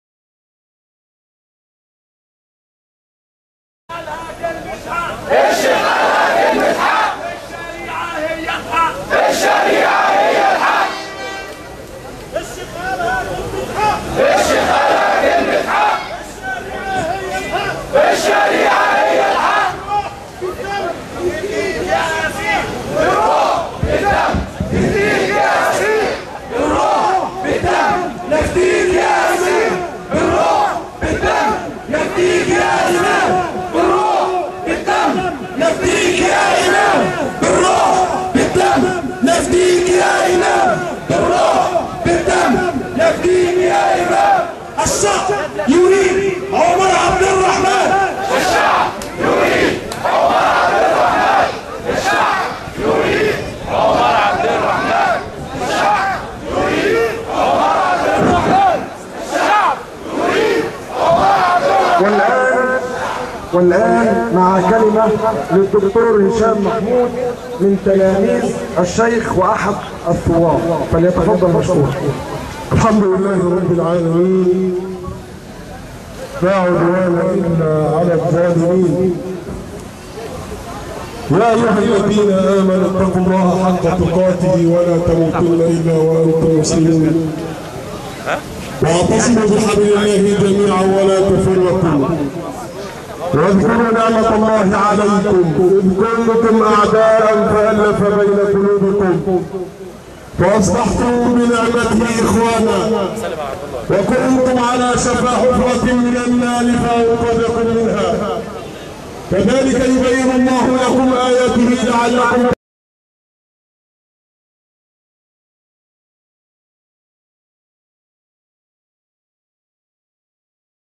إعتصام أمام السفارة الأمريكية للإفراج عن عمرعبدالرحمن
عصر يوم الجمعة إعتصام أمام السفارة الأمريكية للإفراج عن الشيخ عمر عبد الرحمن المسجون بالسجون الأمريكية